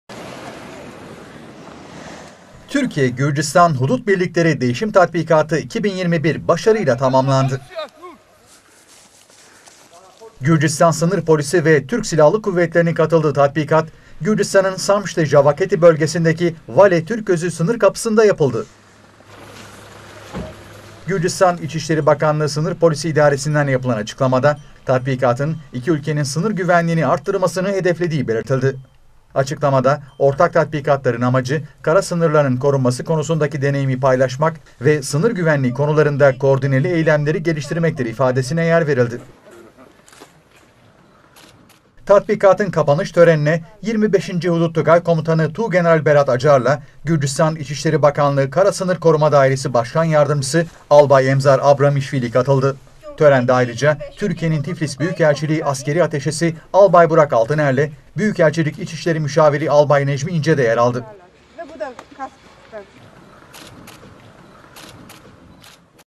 Türkiye ve Gürcistan'dan ortak tatbikat
Türkiye ve Gürcistan ortak sınır tatbikatı yaptı. İki ülke sınır güvenliğinin arttırılmasını hedefleyen tatbikata, Gürcistan ev sahipliği yaptı.